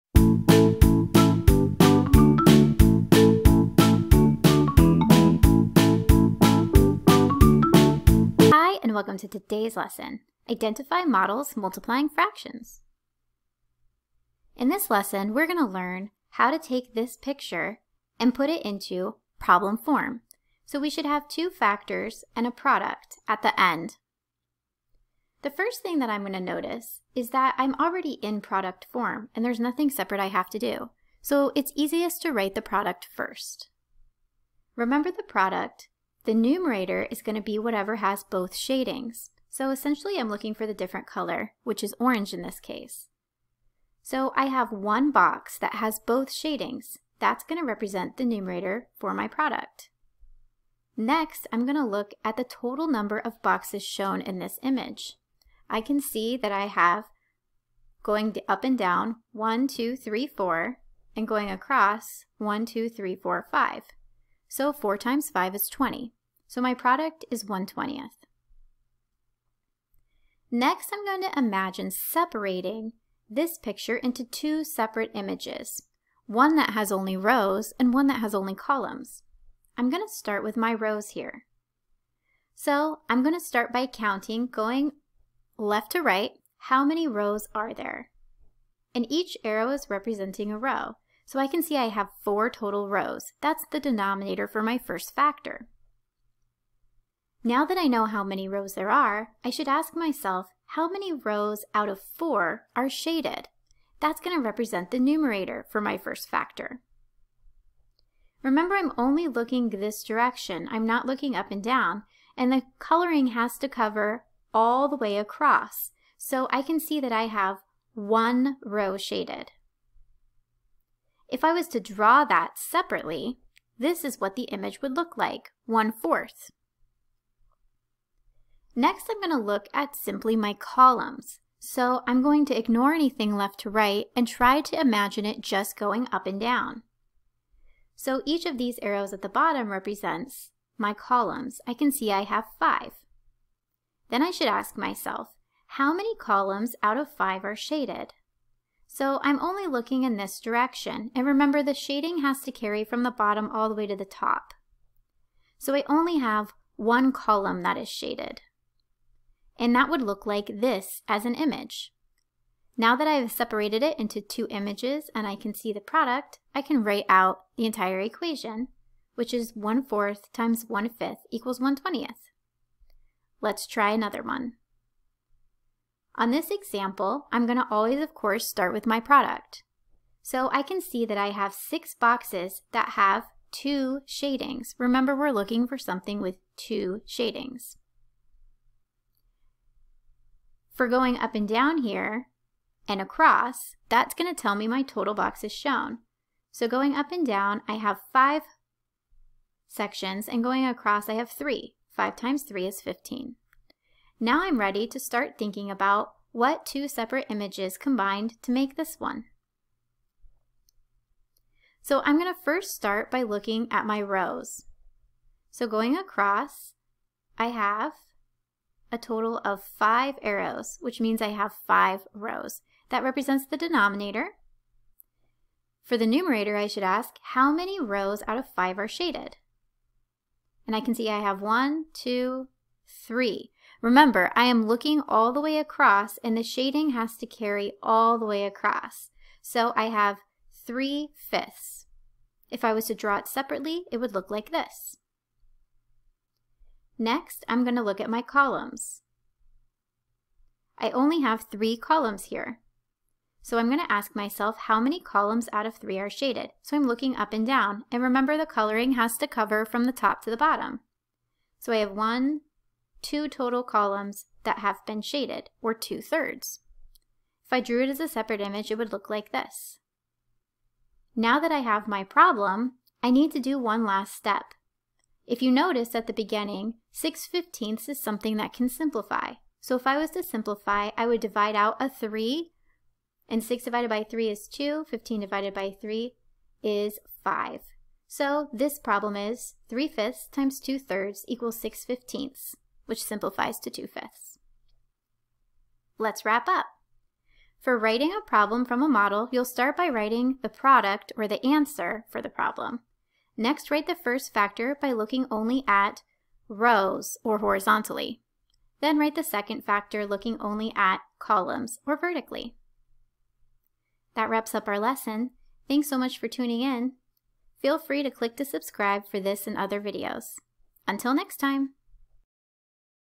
Multiply Fractions: Identifying Models
This lesson explains how to identify the equation used by looking at just the model of the product.  Provides tips for how to visualize each factor separately.